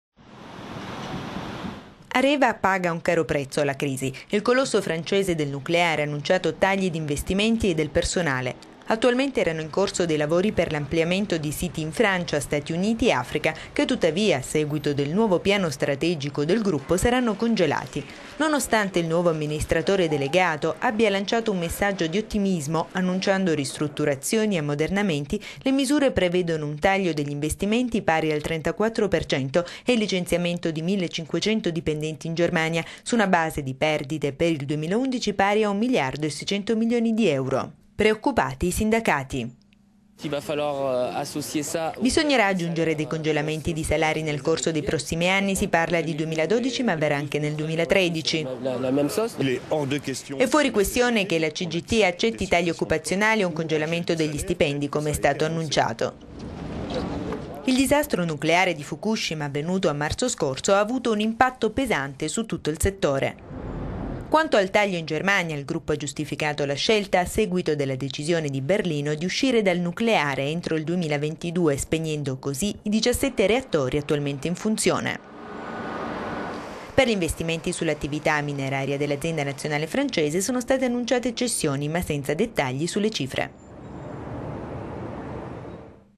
Il piano strategico di Areva per salvarsi dalla crisi
Areva paga un caro prezzo alla crisi. Il colosso francese del nucleare ha annunciato tagli di investimenti e del personale. Attualmente erano in corso dei lavori per l'ampliamento di siti in Francia, Stati Uniti e Africa che, tuttavia, a seguito del nuovo piano strategico del gruppo  saranno congelati.